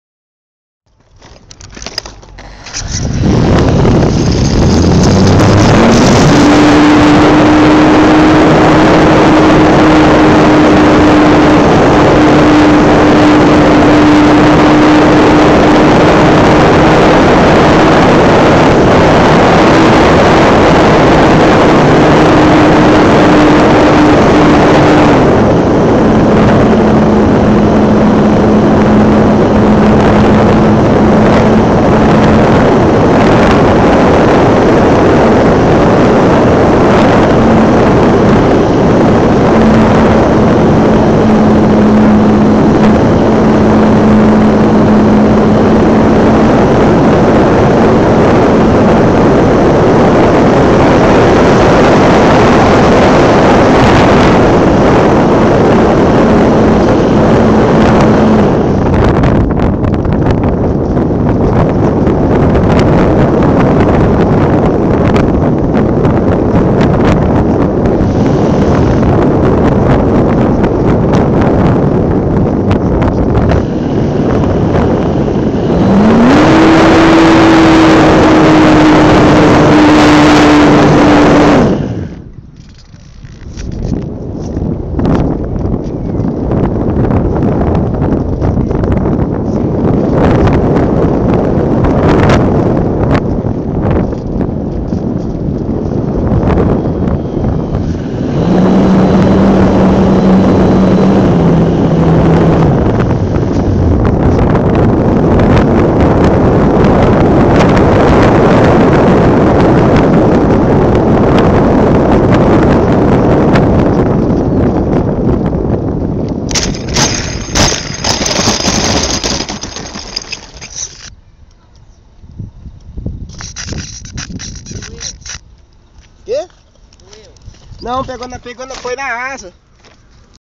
AEROMODELISMO PITANGUI -Voo Panoramico 2-
Voo panorâmico com celular no aeromodelo sobre Pitangui-MG